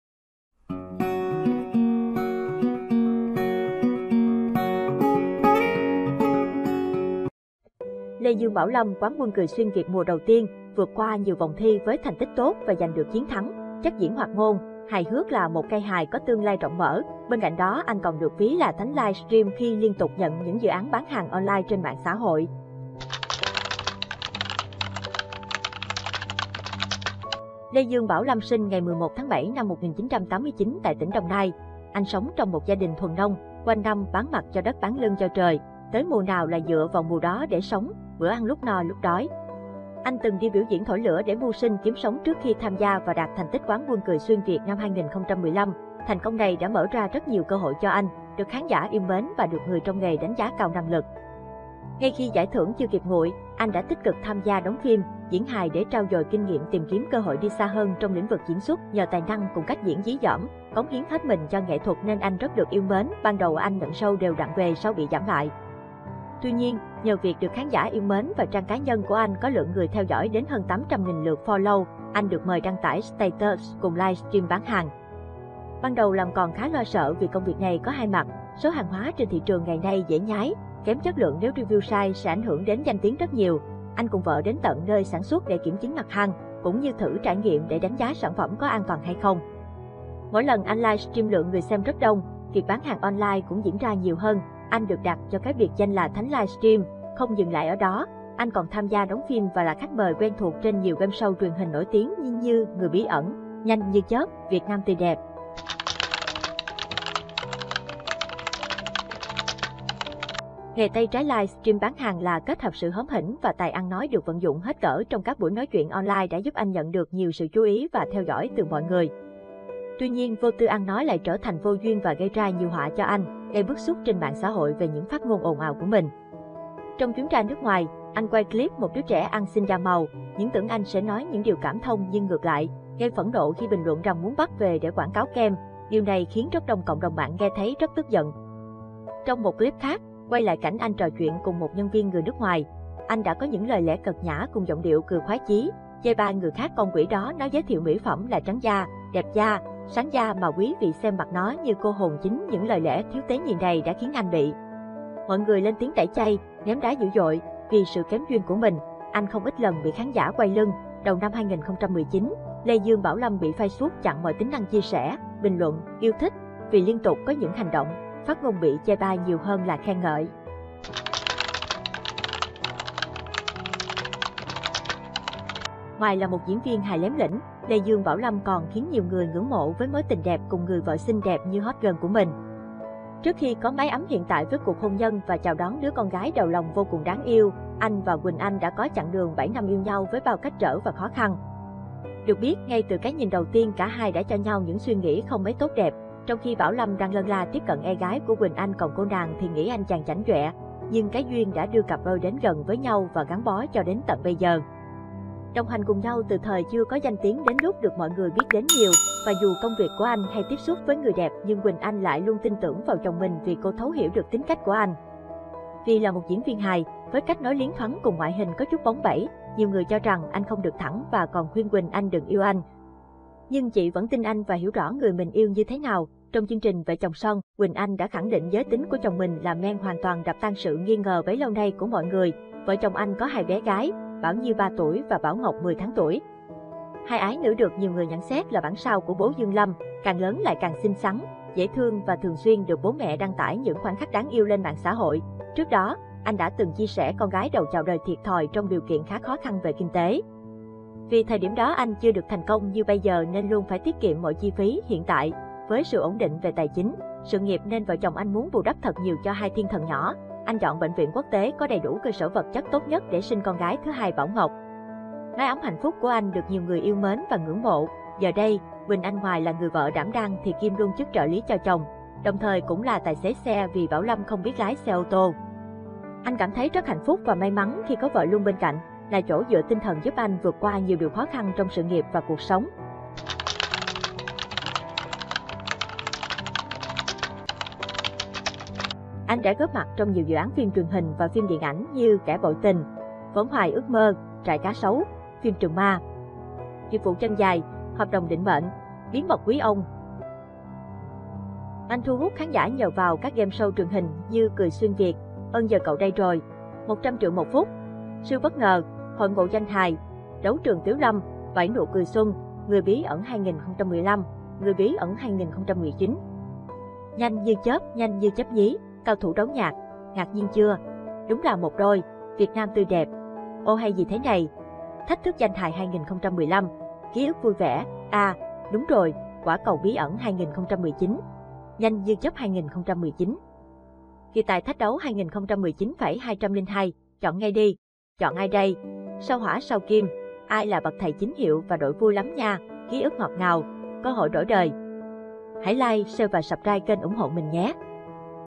Anh Dương Lâm Đồng Nai_Thánh Livestream
Lê Dương Bảo Lâm sinh ngày 11 tháng 7 năm 1989 tại tỉnh Đồng Nai. Anh sống trong một gia đình thuần nông, quanh năm bán mặt cho đất bán lưng cho trời, tới mùa nào là dựa vào mùa đó để sống, bữa ăn lúc no lúc đói. Anh từng đi biểu diễn thổi lửa để mưu sinh, kiếm sống trước khi tham gia và đạt thành tích Quán quân "Cười xuyên Việt" năm 2015. Thành công này đã mở ra rất nhiều cơ hội cho anh, được khán giả yêu mến và được người trong nghề đánh giá cao năng lực. Ngay khi giải thưởng chưa kịp nguội, anh đã tích cực tham gia đóng phim, diễn hài để trau dồi kinh nghiệm tìm kiếm cơ hội đi xa hơn trong lĩnh vực diễn xuất. Nhờ tài năng cùng cách diễn dí dỏm, cống hiến hết mình cho nghệ thuật nên anh rất được yêu mến. Ban đầu anh nhận show đều đặn về sau bị giảm lại. Tuy nhiên, nhờ việc được khán giả yêu mến và trang cá nhân của anh có lượng người theo dõi đến hơn 800.000 lượt follow, anh được mời đăng tải status cùng livestream bán hàng. Ban đầu làm còn khá lo sợ vì công việc này có hai mặt, số hàng hóa trên thị trường ngày nay dễ nhái, kém chất lượng nếu review sai sẽ ảnh hưởng đến danh tiếng rất nhiều. Anh cùng vợ đến tận nơi sản xuất để kiểm chứng mặt hang, cũng như thử trải nghiệm để đánh giá sản phẩm có an toàn hay không. Mỗi lần anh livestream lượng người xem rất đông, việc bán hàng online cũng diễn ra nhiều hơn, anh được đặt cho cái biệt danh là “thánh livestream “. Không dừng lại ở đó, anh còn tham gia đóng phim và là khách mời quen thuộc trên nhiều gameshow truyền hình nổi tiếng như: Người bí ẩn, Nhanh như chớp, Việt Nam tươi đẹp
Ngoài là một diễn viên hài lém lỉnh, Lê Dương Bảo Lâm còn khiến nhiều người ngưỡng mộ với mối tình đẹp cùng người vợ xinh đẹp như hot girl của mình. Trước khi có mái ấm hiện tại với cuộc hôn nhân và chào đón đứa con gái đầu lòng vô cùng đáng yêu, anh và Quỳnh Anh đã có chặng đường 7 năm yêu nhau với bao cách trở và khó khăn. Được biết, ngay từ cái nhìn đầu tiên cả hai đã cho nhau những suy nghĩ không mấy tốt đẹp, trong khi Bảo Lâm đang lân la tiếp cận e gái của Quỳnh Anh còn cô nàng thì nghĩ anh chàng chảnh chọe. Nhưng cái duyên đã đưa cặp đôi đến gần với nhau và gắn bó cho đến tận bây giờ. Đồng hành cùng nhau từ thời chưa có danh tiếng đến lúc được mọi người biết đến nhiều, và dù công việc của anh hay tiếp xúc với người đẹp nhưng Quỳnh Anh lại luôn tin tưởng vào chồng mình vì cô thấu hiểu được tính cách của anh. Vì là một diễn viên hài, với cách nói liến thoắng cùng ngoại hình có chút bóng bẩy, nhiều người cho rằng anh"không được thẳng" và còn khuyên Quỳnh Anh đừng yêu anh. Nhưng chị vẫn tin anh và hiểu rõ người mình yêu như thế nào, trong chương trình Vợ Chồng Son, Quỳnh Anh đã khẳng định giới tính của chồng mình là men hoàn toàn đập tan sự nghi ngờ bấy lâu nay của mọi người. Vợ chồng anh có 2 bé gái, Bảo Nhi 3 tuổi và Bảo Ngọc 10 tháng tuổi. Hai ái nữ được nhiều người nhận xét là “bản sao” của bố Dương Lâm. Càng lớn lại càng xinh xắn, dễ thương và thường xuyên được bố mẹ đăng tải những khoảnh khắc đáng yêu lên mạng xã hội. Trước đó, anh đã từng chia sẻ con gái đầu chào đời thiệt thòi trong điều kiện khá khó khăn về kinh tế. Vì thời điểm đó anh chưa được thành công như bây giờ nên luôn phải tiết kiệm mọi chi phí. Hiện tại, với sự ổn định về tài chính, sự nghiệp nên vợ chồng anh muốn bù đắp thật nhiều cho hai thiên thần nhỏ. Anh chọn bệnh viện quốc tế có đầy đủ cơ sở vật chất tốt nhất để sinh con gái thứ hai Bảo Ngọc. Mái ấm hạnh phúc của anh được nhiều người yêu mến và ngưỡng mộ. Giờ đây, Quỳnh Anh ngoài là người vợ đảm đang thì kiêm luôn chức trợ lý cho chồng, đồng thời cũng là tài xế xe vì Bảo Lâm không biết lái xe ô tô. Anh cảm thấy rất hạnh phúc và may mắn khi có vợ luôn bên cạnh, là chỗ dựa tinh thần giúp anh vượt qua nhiều điều khó khăn trong sự nghiệp và cuộc sống.